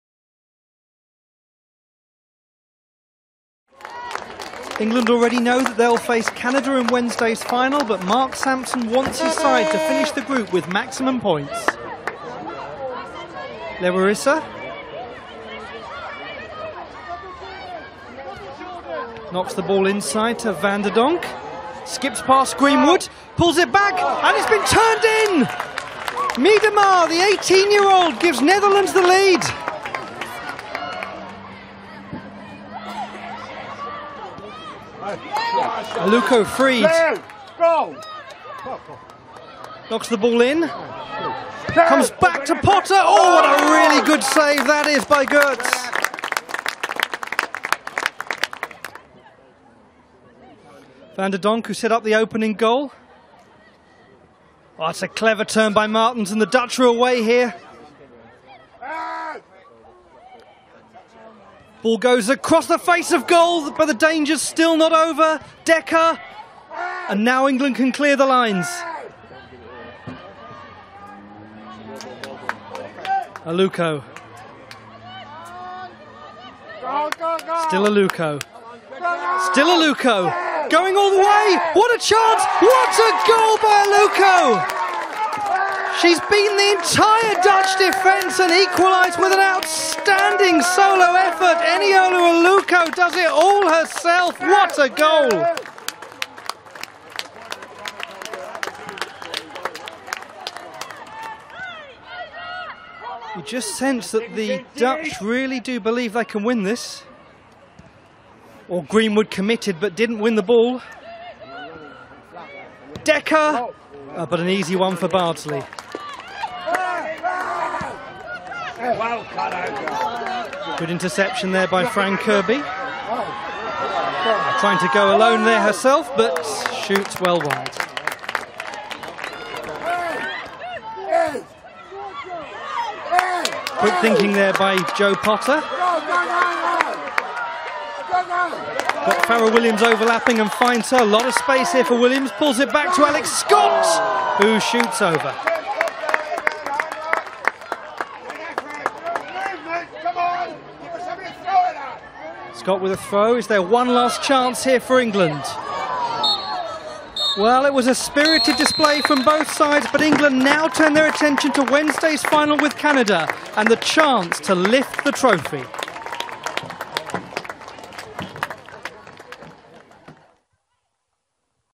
England Women 1-1 Holland - Cyprus Cup | Goals & Highlights
Eniola Aluko scored a memorable goal to earn England a draw against Holland in the final group match at the Cyprus Cup.

Mark Sampson’s side had already booked their place in the final